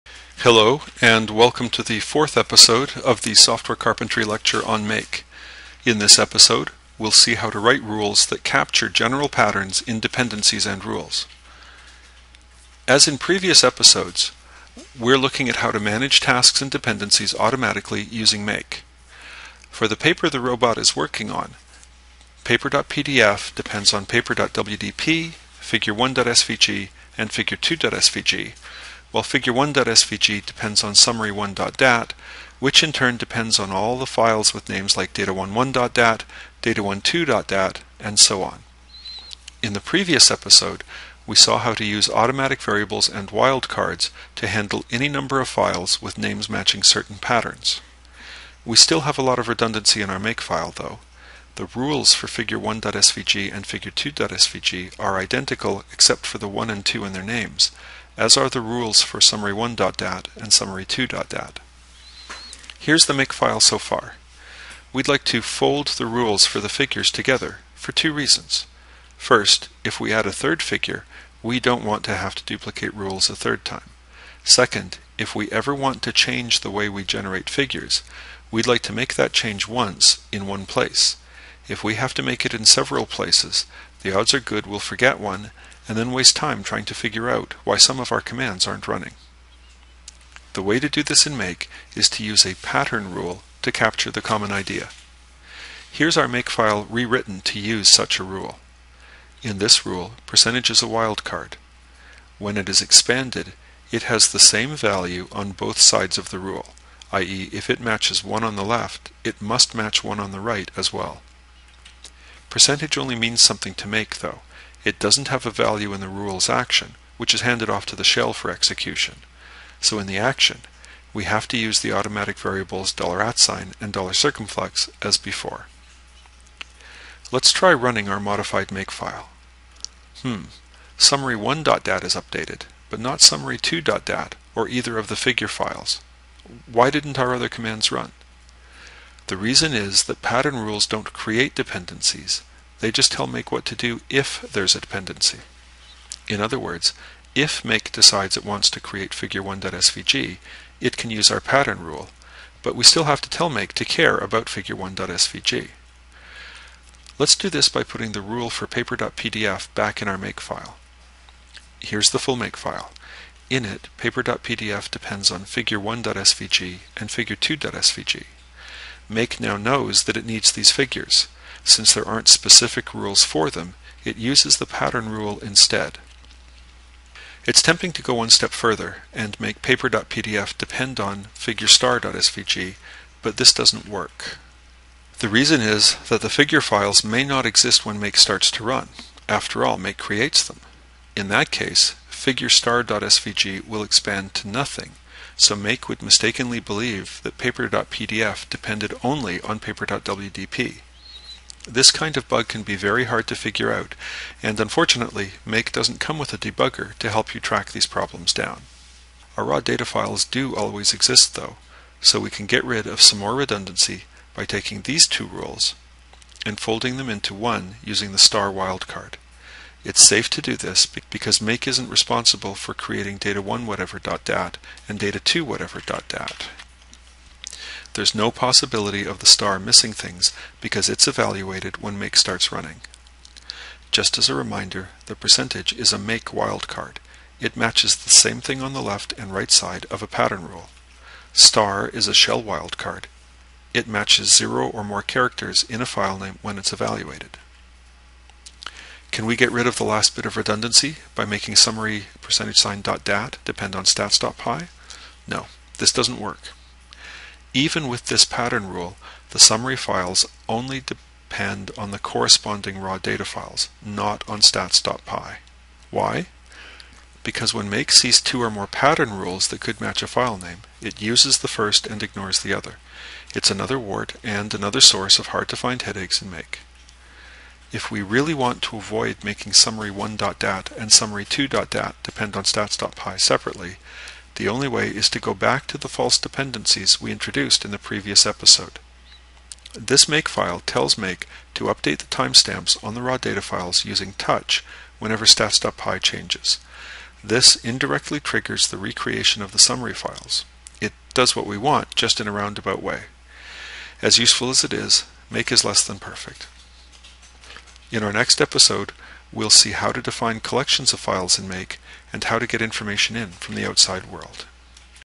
Make - Episode 4 - Rules
Welcome to the Software Carpentry lecture on Make. In this episode, we'll see how to write rules that capture general patterns in dependencies and rules.